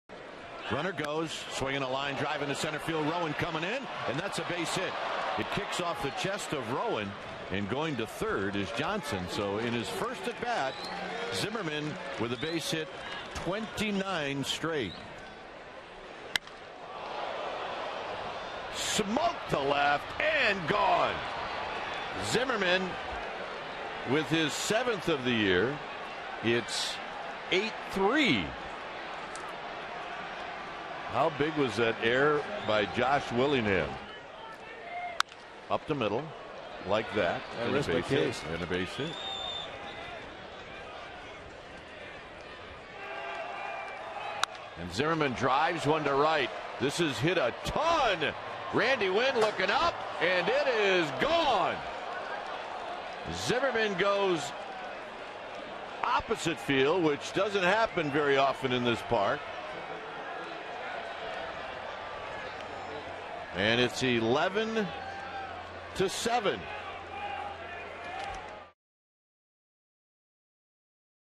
2009/05/11 Zimmerman's four-hit game
WSH@SF: Zimmerman colects four hits, four RBIs